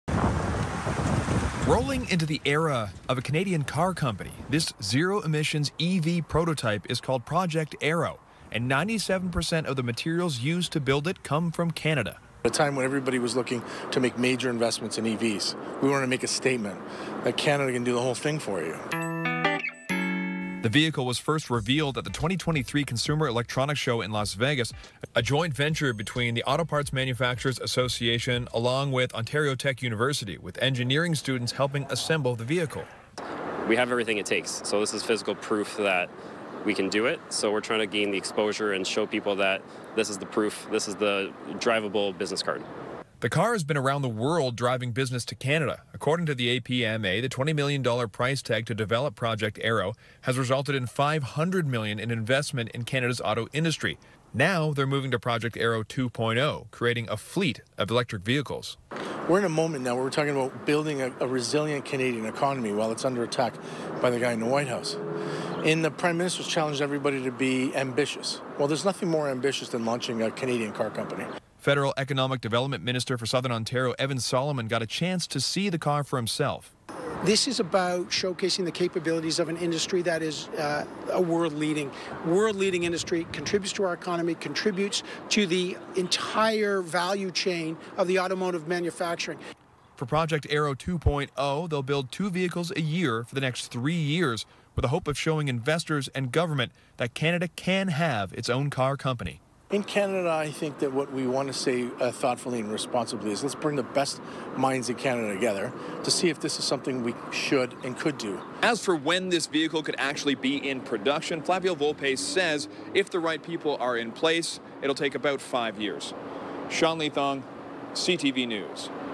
Meet the nearly all-Canadian made zero-emission electric car assembled by Ontario Tech students
Sean Leathong has more on a zero-emission electric car made with nearly all-Canadian components assembled by students at Ontario Tech